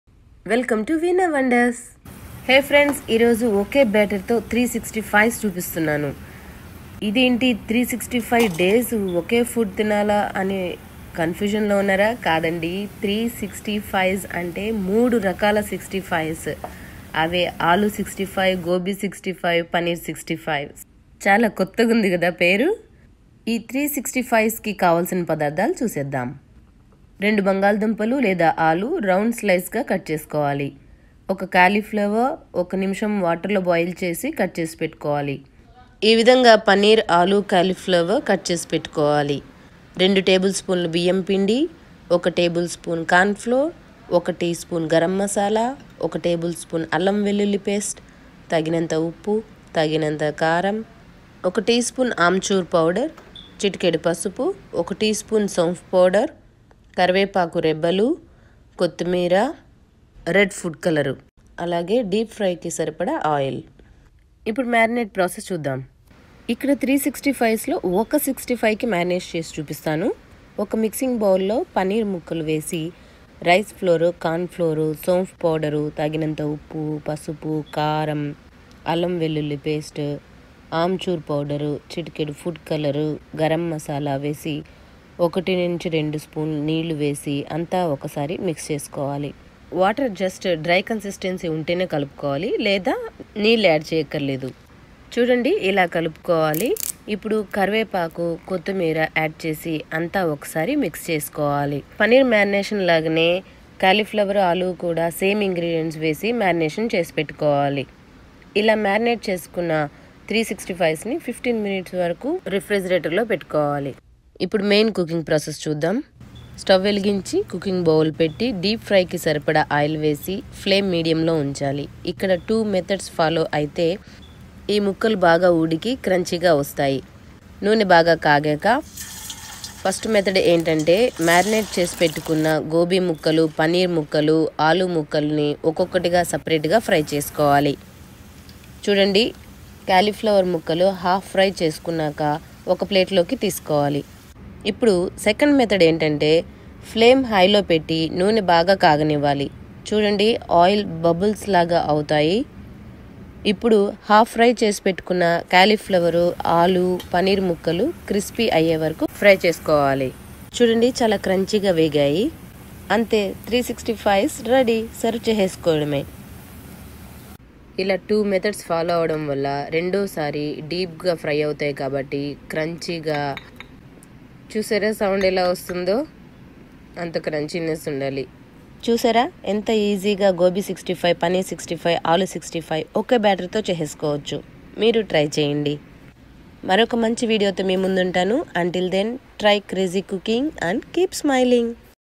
పర్ఫెక్ట్ గోబీ 65,paneer 65,ఆలూ 65 ఒక్క మిస్రమముతోనే,Gobi65,paneer 65,aloo65 only one batter mix
These 3’65’s (paneer 65,gobi65,aloo 65only with one batter mix can make it easily and with perfect following methods.while biting gives nice crunchy taste.While marinations,Aroma smells good.while eating tummy says cook every day 😋👌🏻